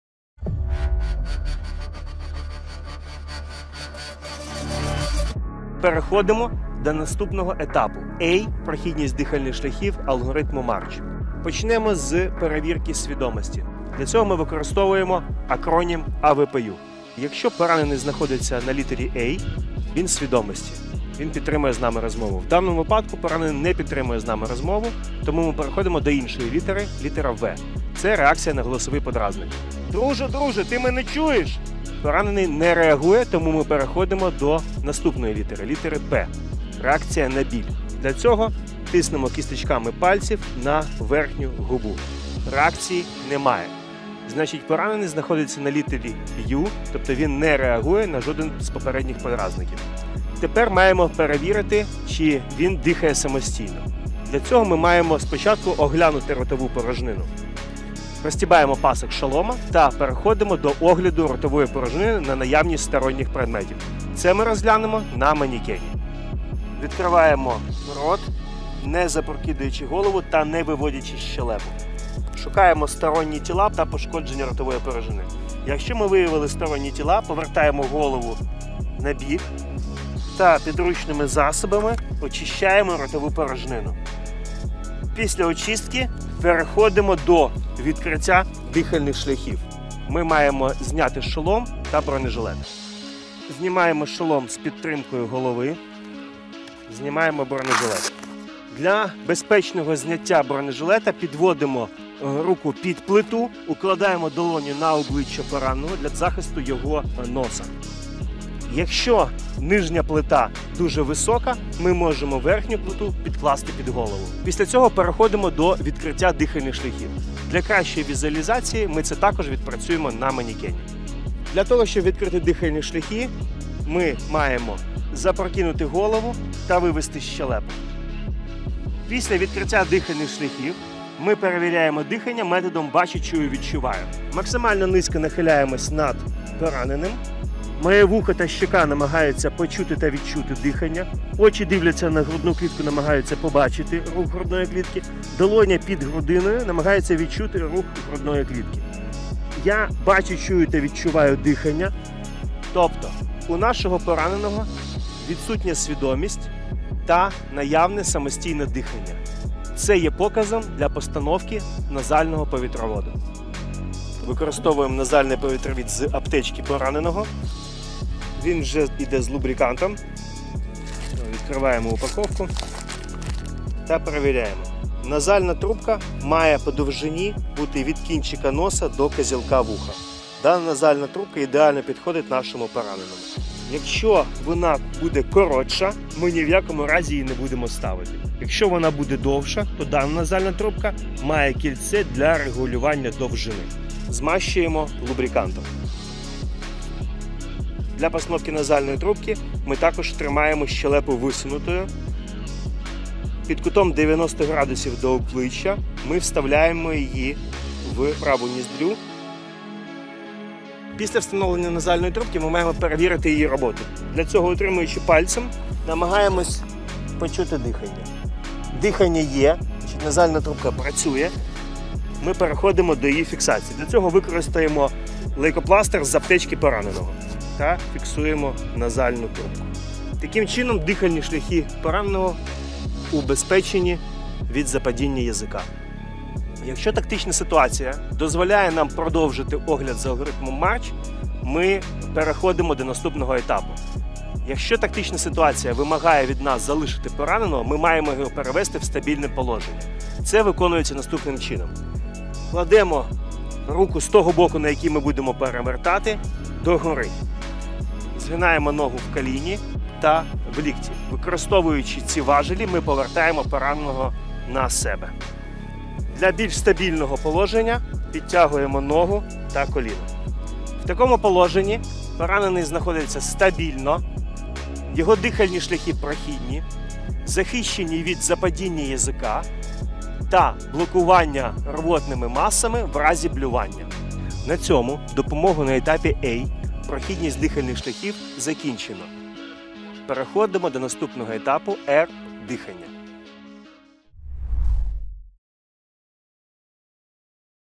Такмед. Лекція 4: Алгоритм MARCH. A - Прохідність дихальних шляхів
Після зупинки масивної кровотечі наступний етап допомоги - перевірка дихання у пораненого та забезпечення прохідності його дихальних шляхів. У цьому відео розглянемо наступні дії:

▪️ визначення рівня свідомості пораненого за шкалою AVPU
▪️ перевірка дихання за методом «Бачу. Чую. Відчуваю»
▪️ відкриття дихальних шляхів
▪️ постановка назофарингеального повітроводу
▪️ переведення пораненого у стабільне положення